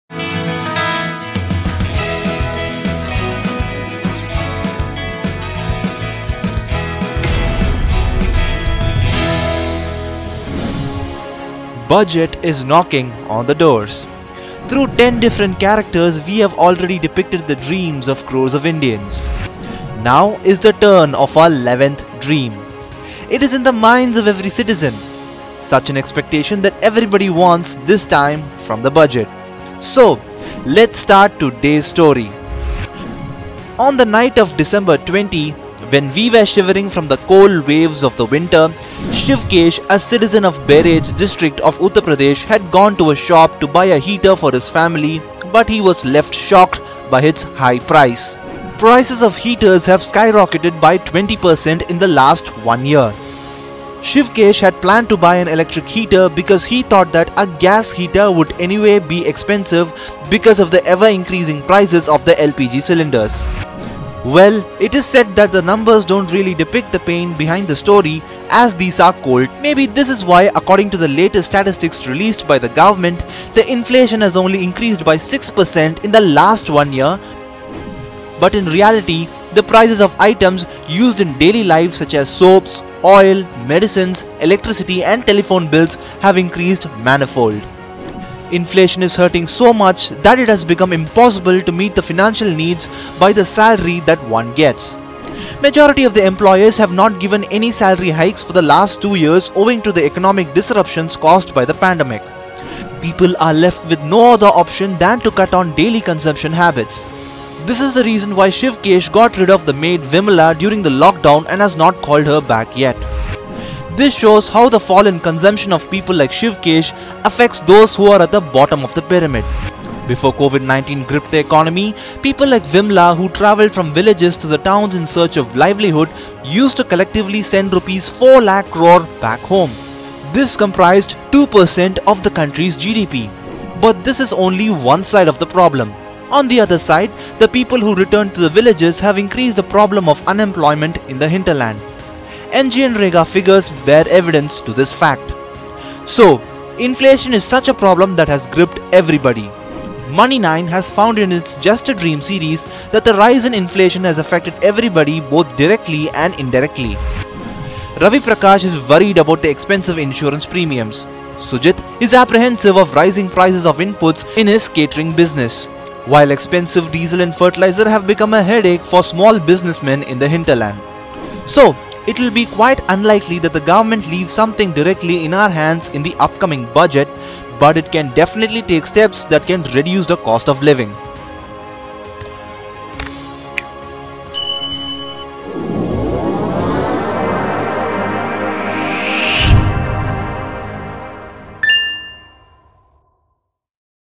Budget special: Will budget 2022-23 suffice inflation
Inflation is hurting so much that it has become impossible to meet the financial needs by the salary that one gets. Majority of employers have not given any salary hikes for the last two years owing to the economic disruptions caused by the pandemic. People are left with no than to cut on daily consumption habits.